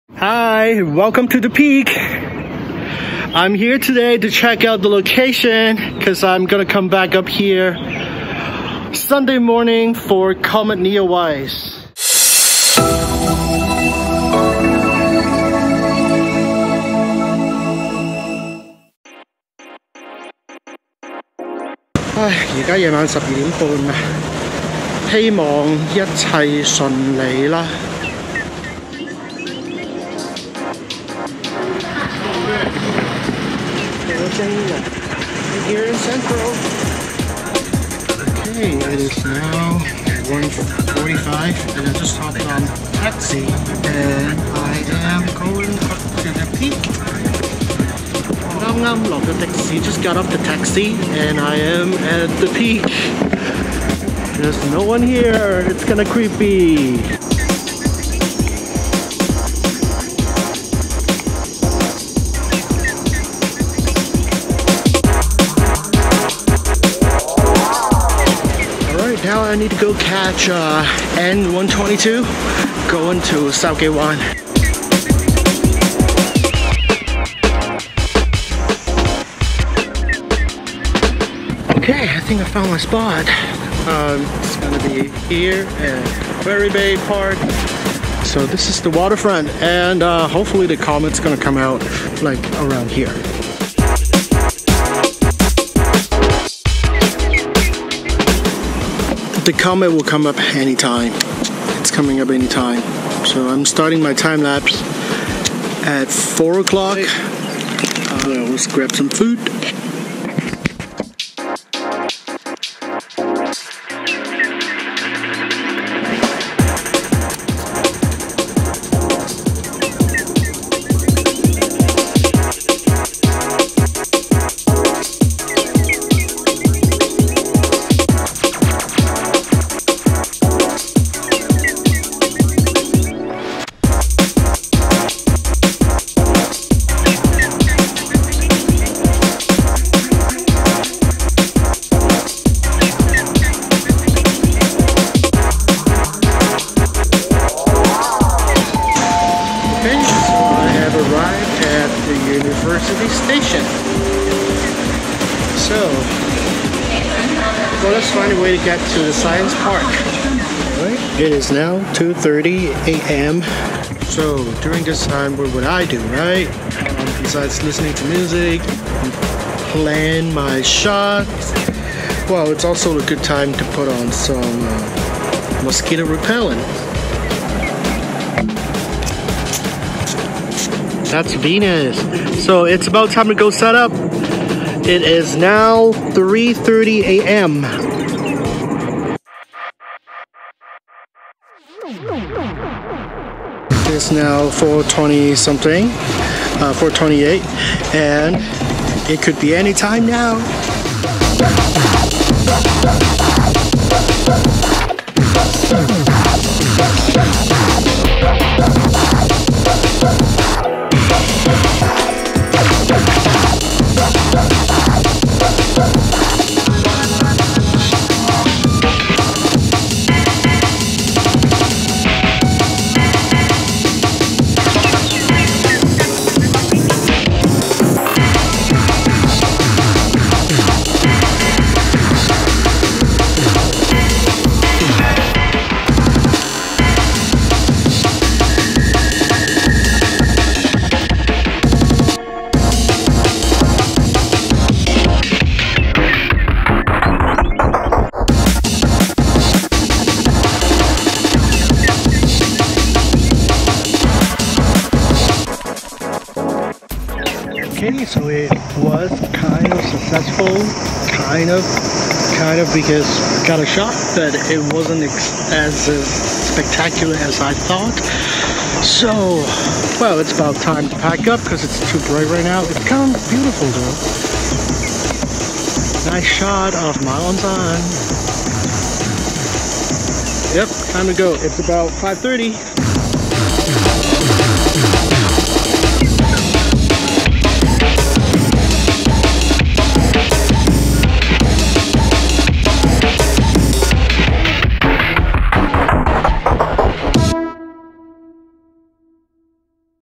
Chasing Comet Neowise C/2020 F3 in Hong Kong
I chased Comet Neowise for three nights... Hong Kong is very low in latitude, so it is extremely difficult to see Comet Neowise... plus the bad weathers~~~ Here's a video of my three nights.